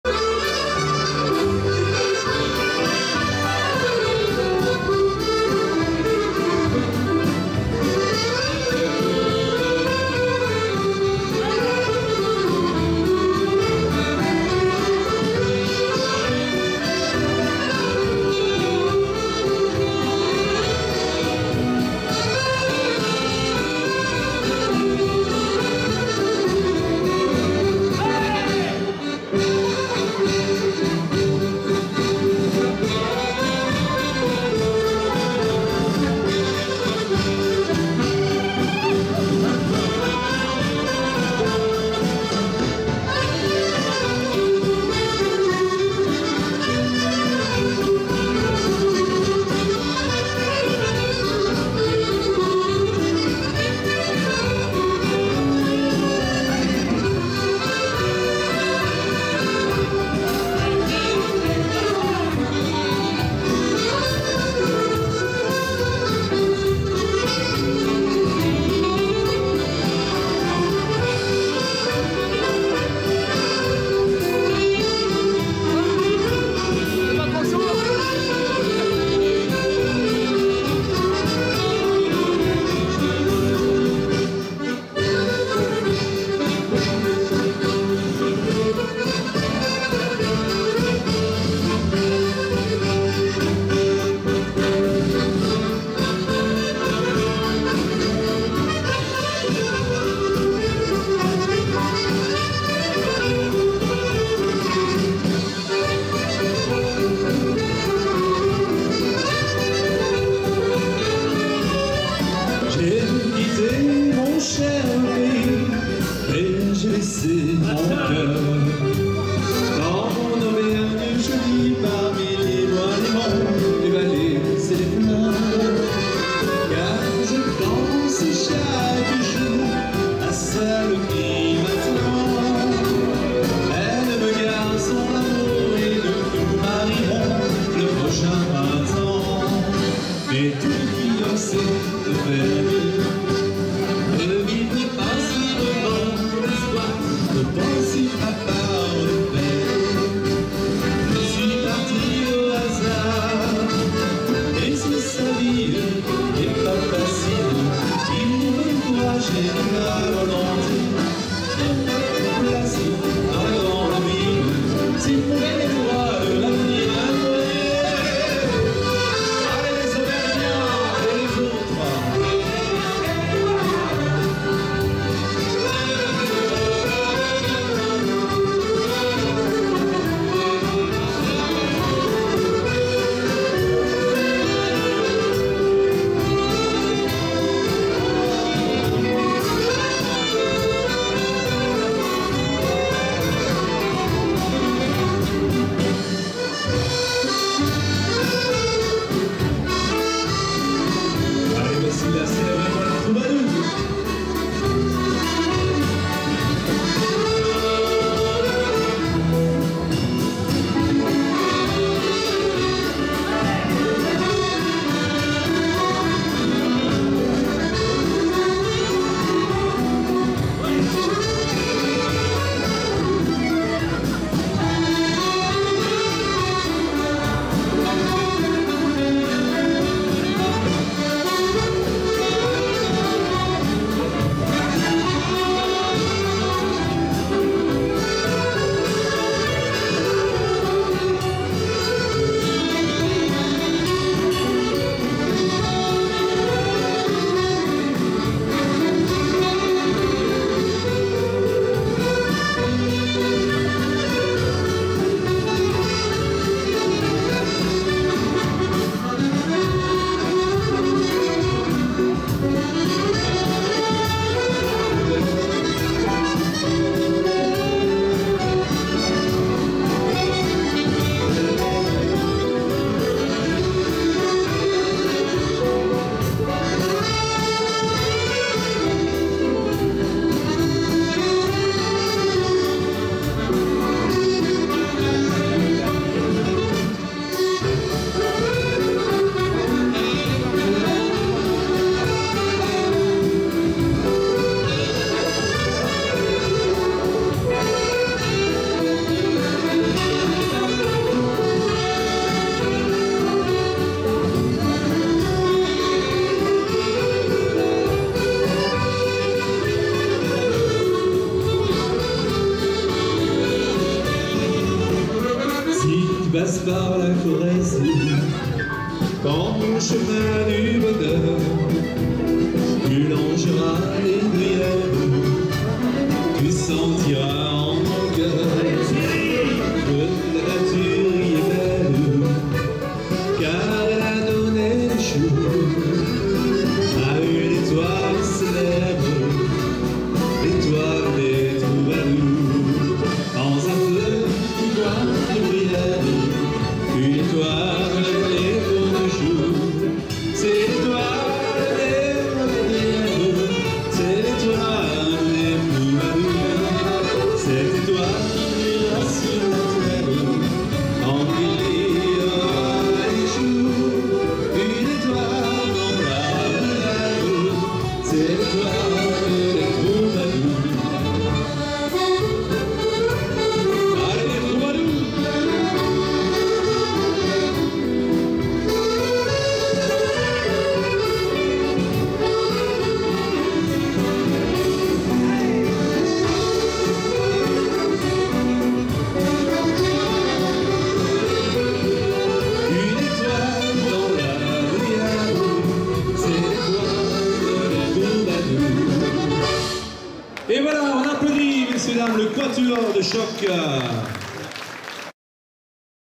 C. ROCHE V. POUGET J. LOCATELLI S. METHOT St Just St Rambert sept 2019 Valses
Festival d'accordéon St Just St Rambert  (42) 15 sept 2019 - organisé et présenté par Mathilde ROYET - Orchestre Virginie POUGET -  vidéo et réalisation Alexandra Astorg (Sandy)